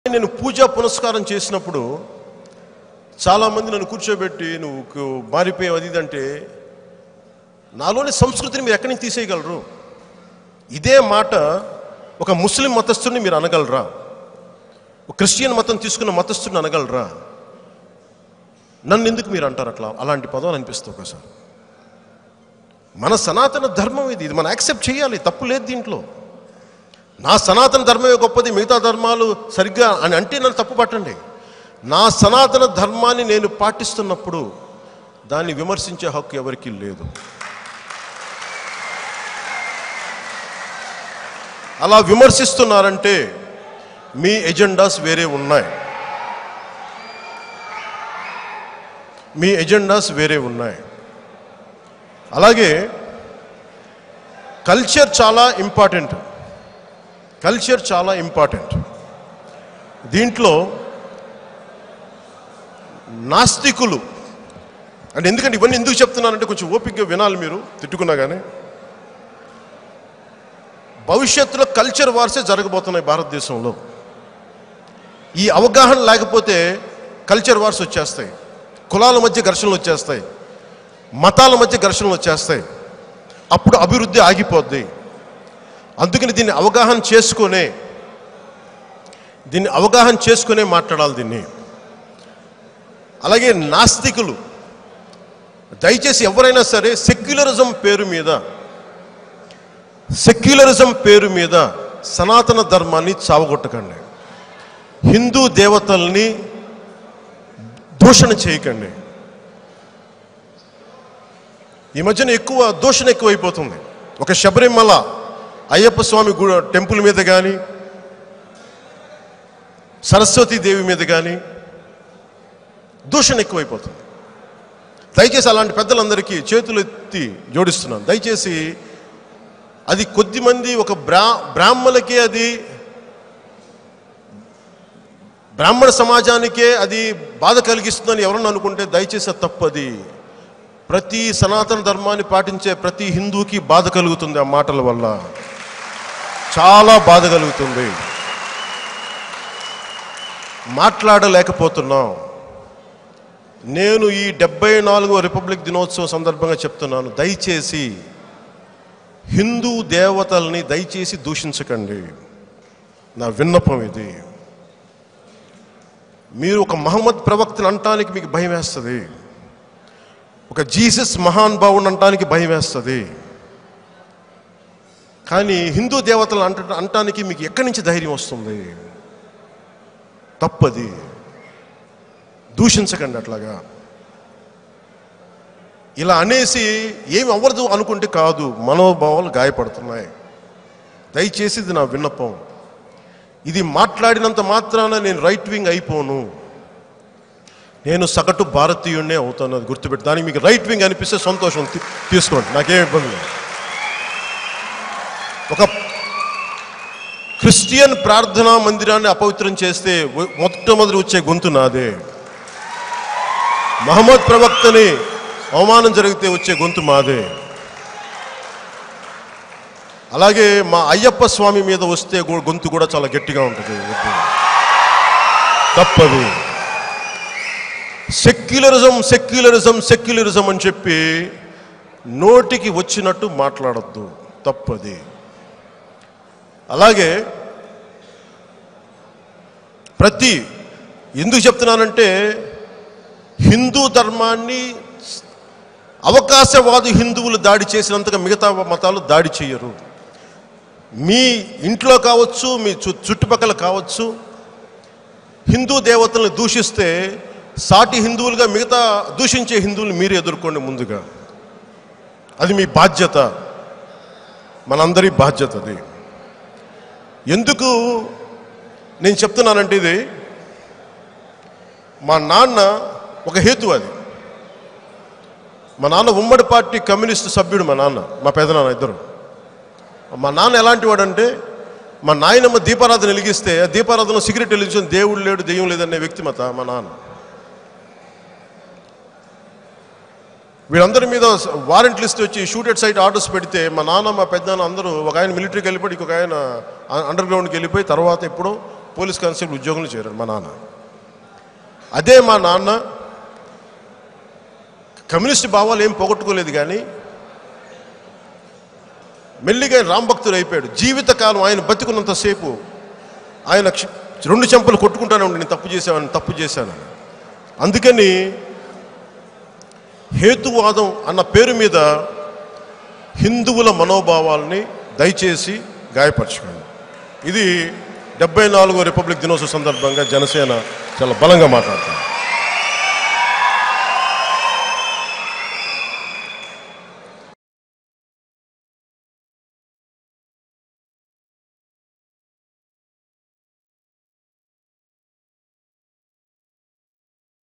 సెక్యులరిజం పేరుతో దేవుళ్ళు , దేవతలను దూషించకండి | Pawan kalyan latest | @teluguvani8
Pawan kalyan about culture | pawan kalyan about secularism | pawankalyan latest @teluguvani8